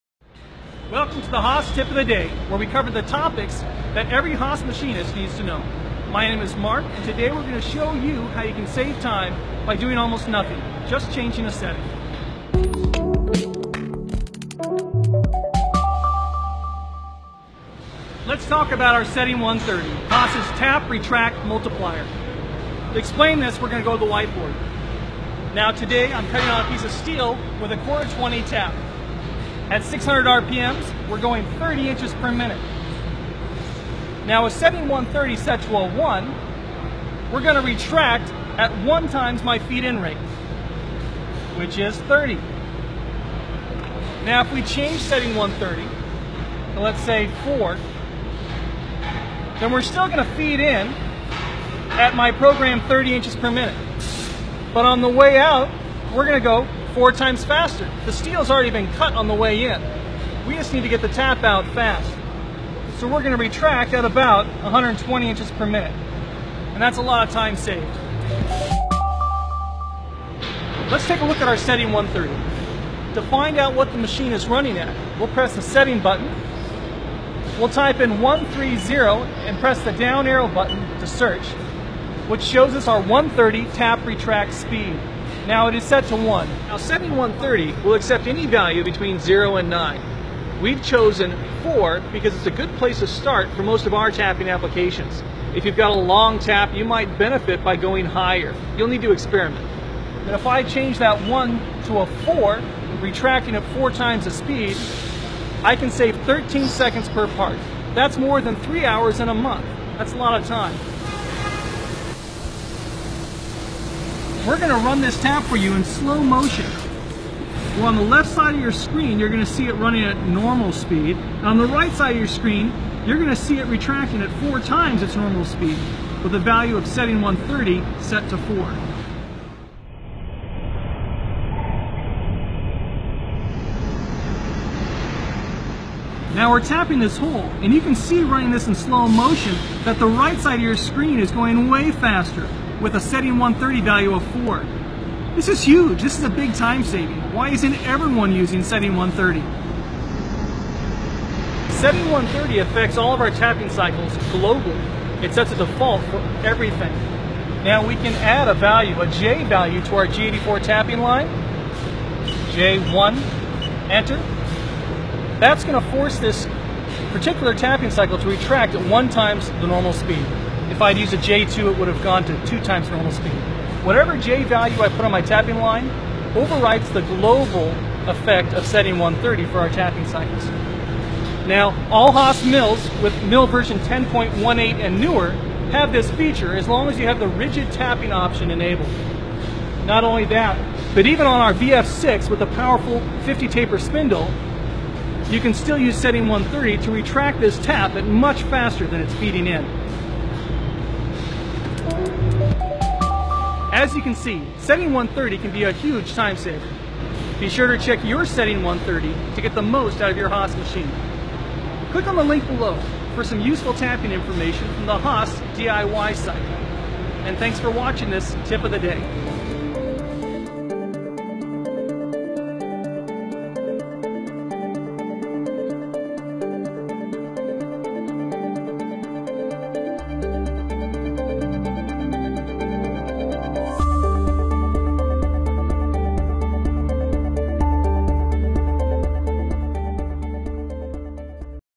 Roscado más rápido con el ajuste 130
Cómo podemos acortar los ciclos de roscado aumentando la velocidad.